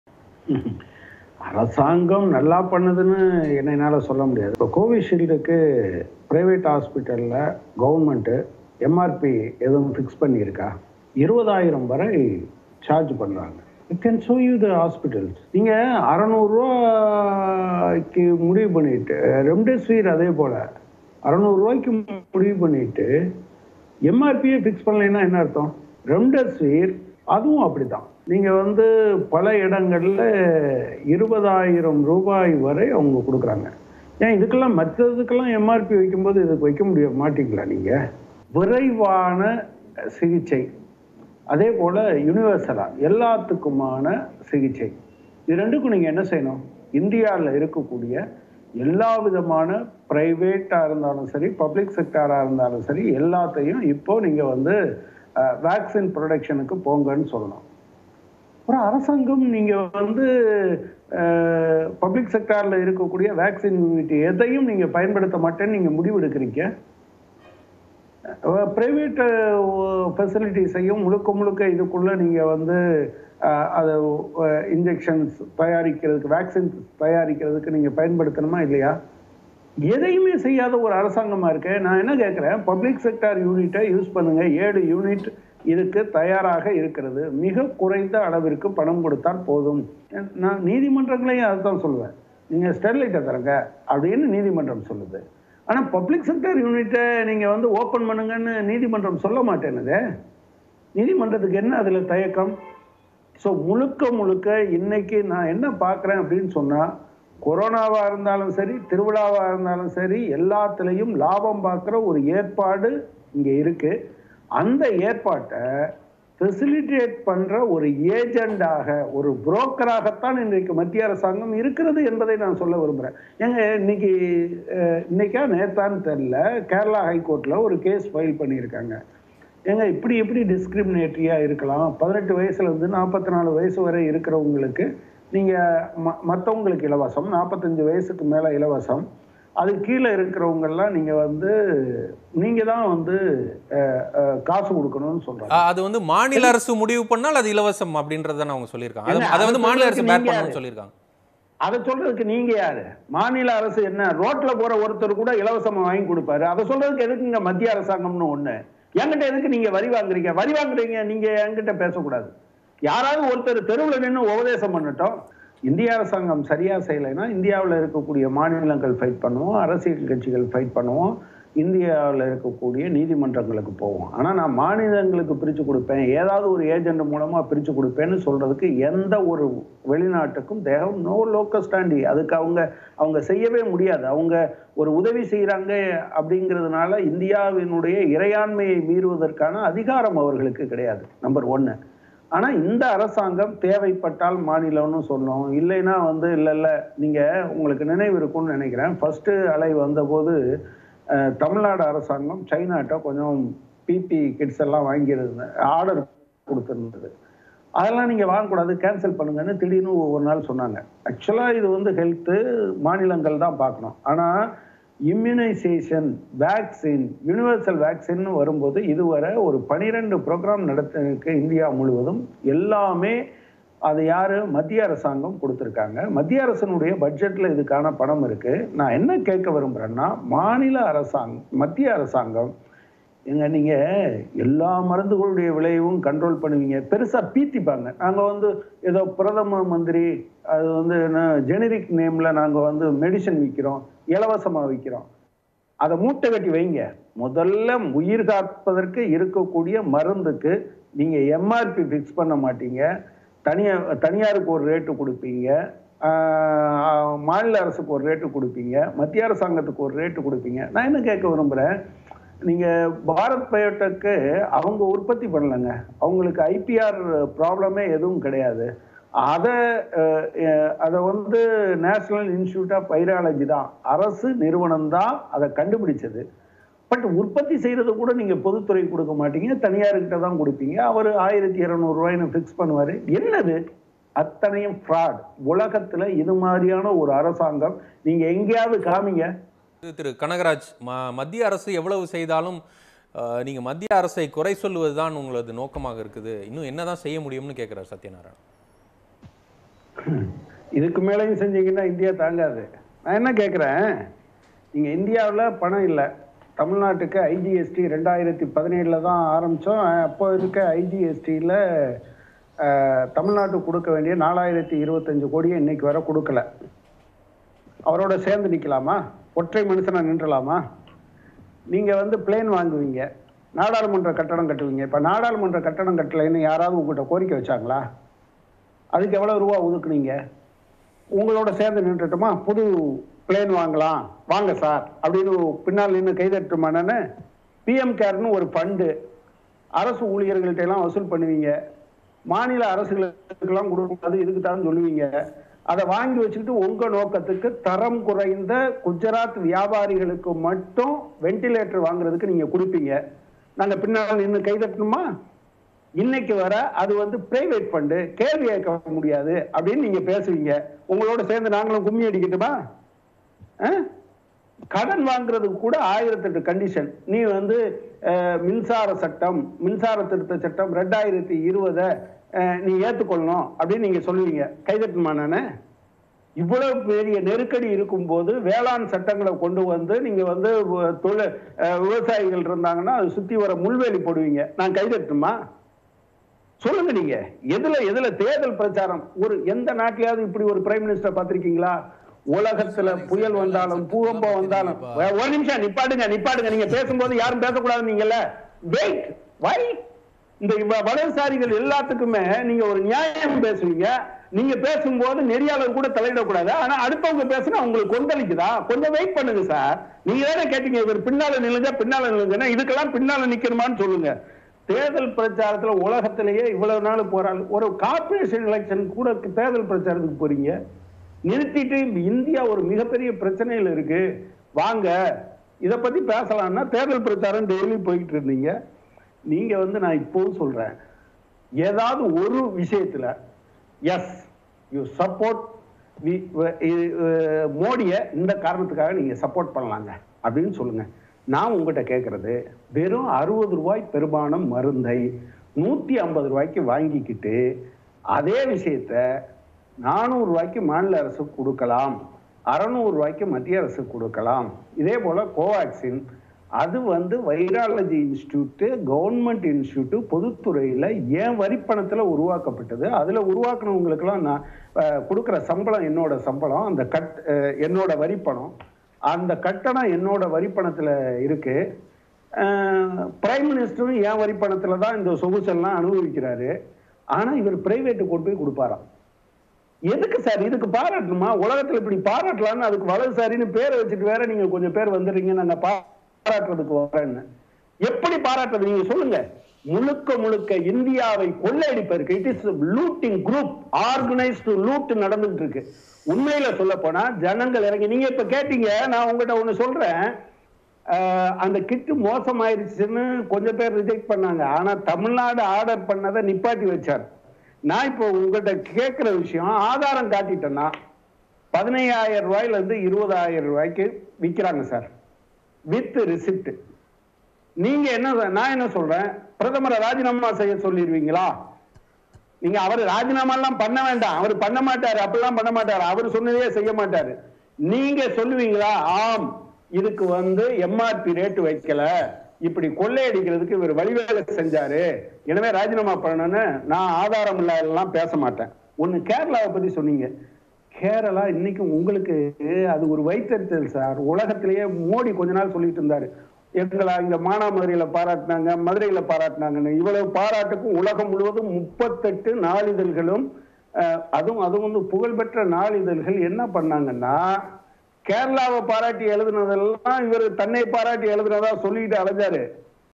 முதலாளிகளுக்கு லாபம் பார்த்து கொடுக்கும் புரோக்கர் மத்திய மோடி அரசு
முதலாளிகளுக்கு லாபம் பார்த்து கொடுக்கும் புரோக்கராக செயல்படுகிறது மத்திய அரசு - தோழர் க.கனகராஜ், மாநில செயற்குழு உறுப்பினர்